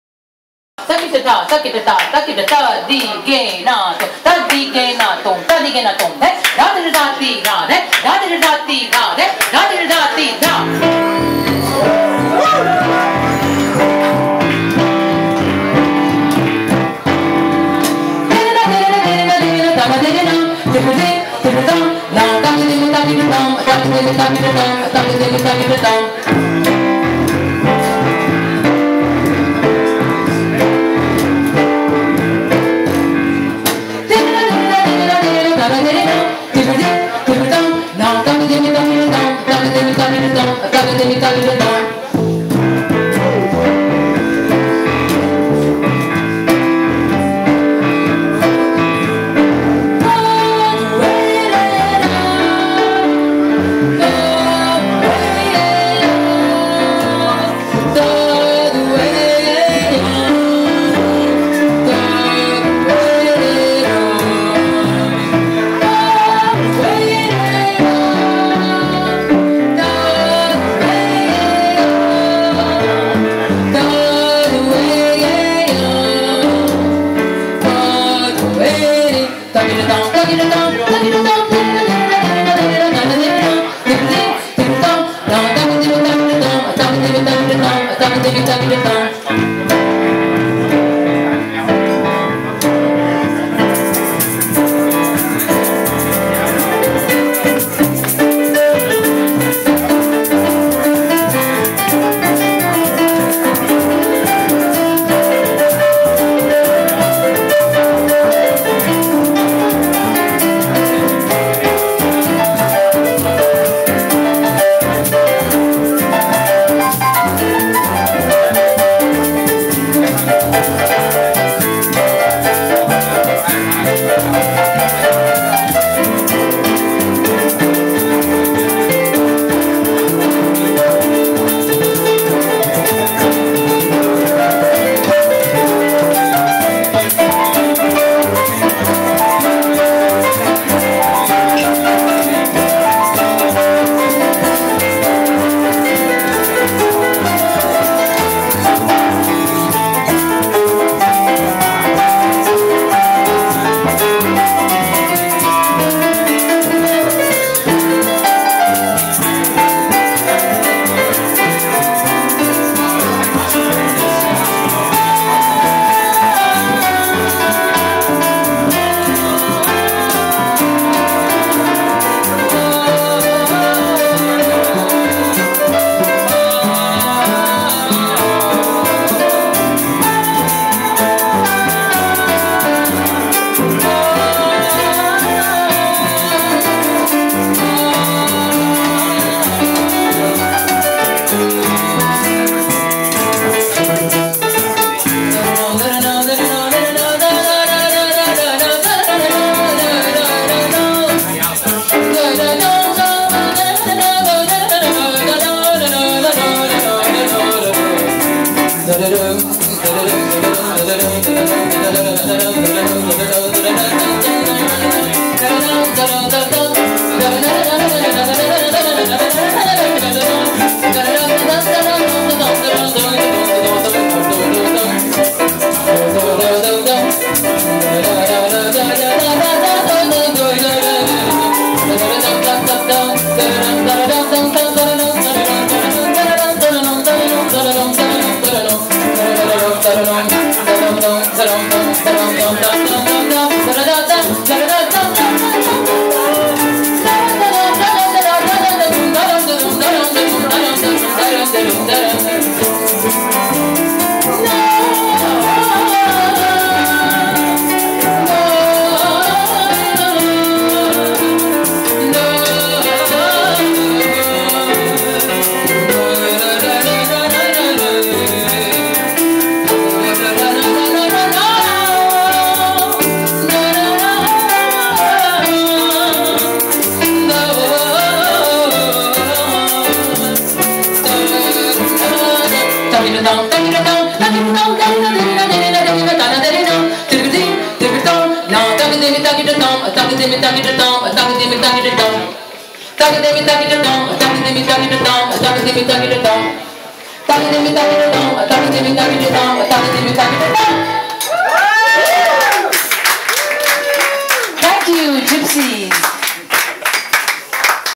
Sandhya Sanjana
With some money and a cellphone in my pocket, I went to Sound Garden.

Sandhya Sanjana was singing there. I could not resist to make a video.